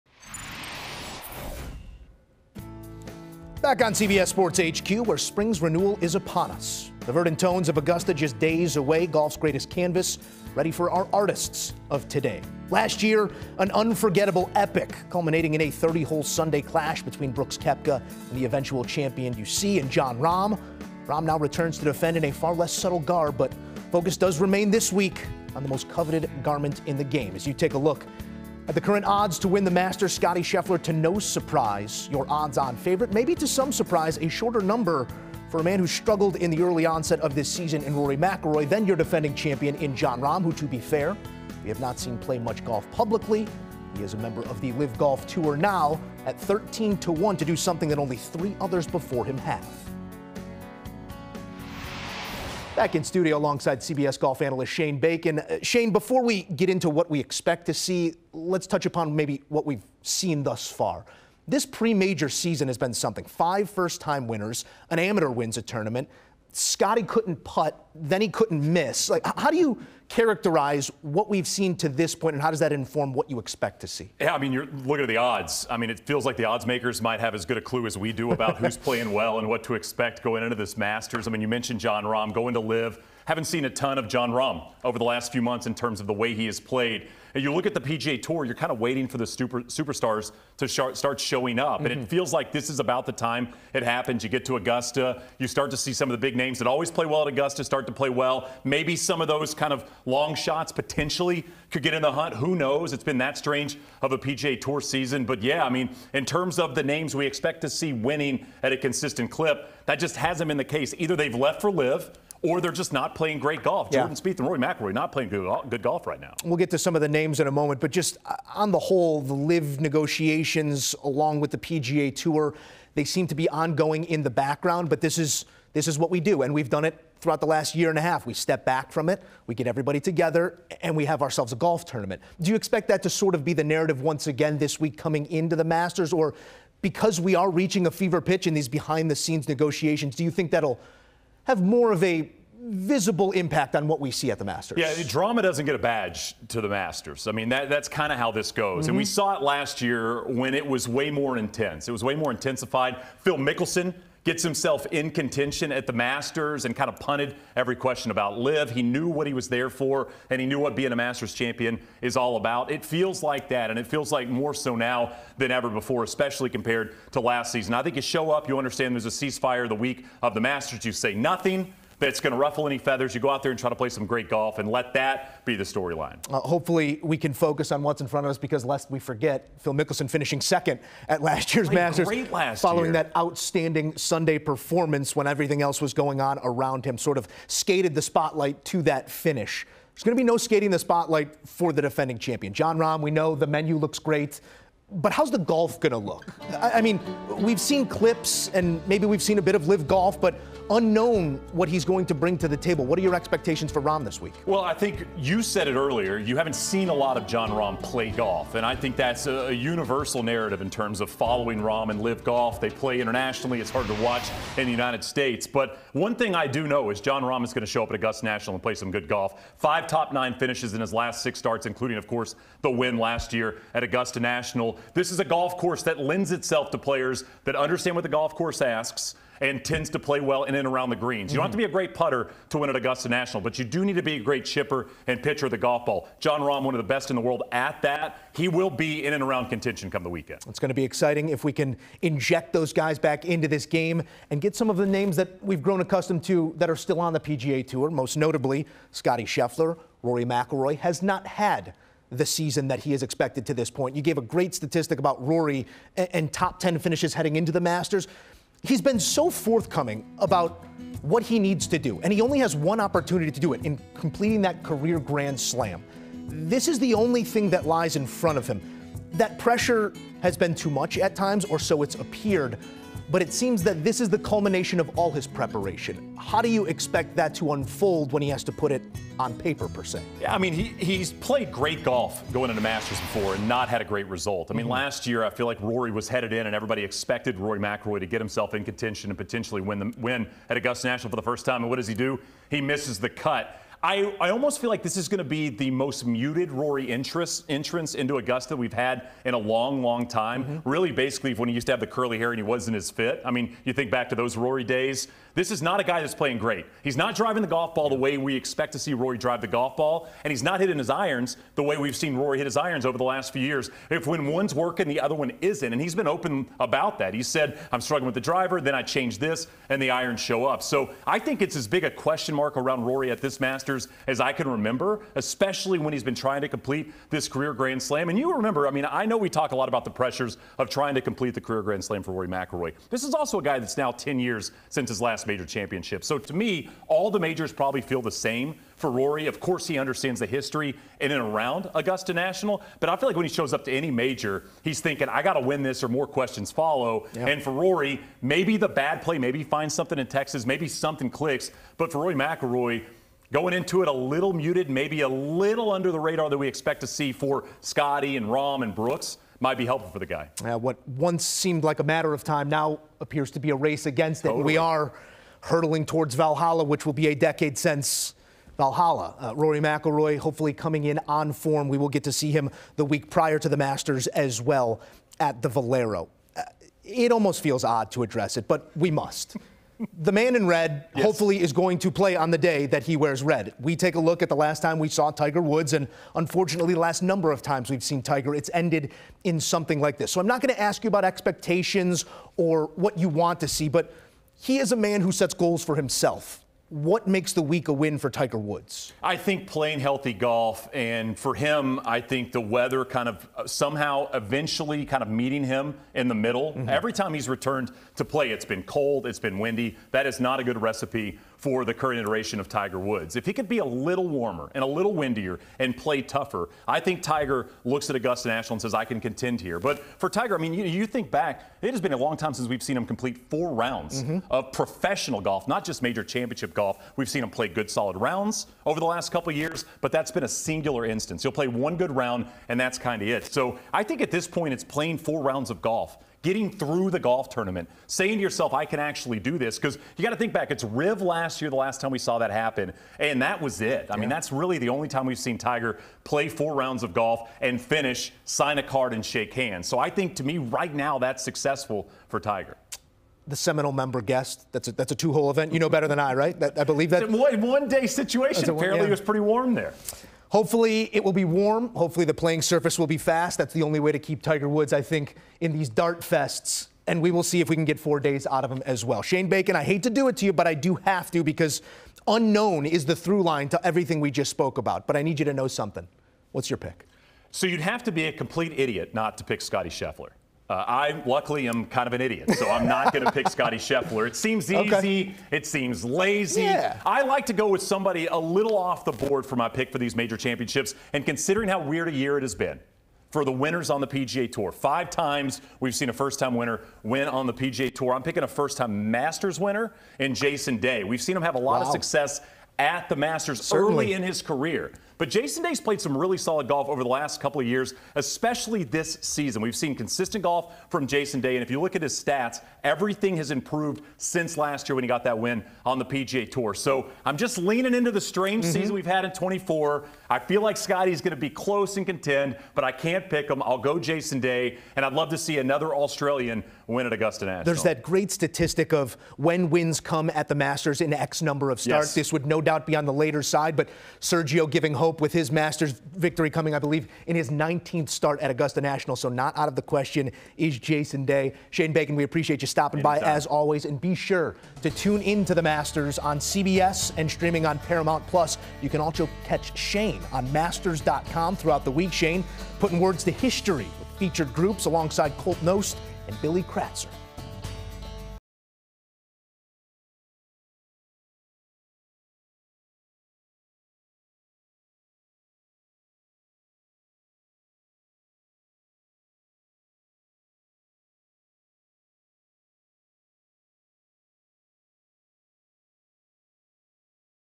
2024 Masters SUPER PREVIEW: BIGGEST Storylines + Pick To Win I CBS Sports
Shane Bacon joins CBS Sports HQ to preview the 2024 Masters Tournament.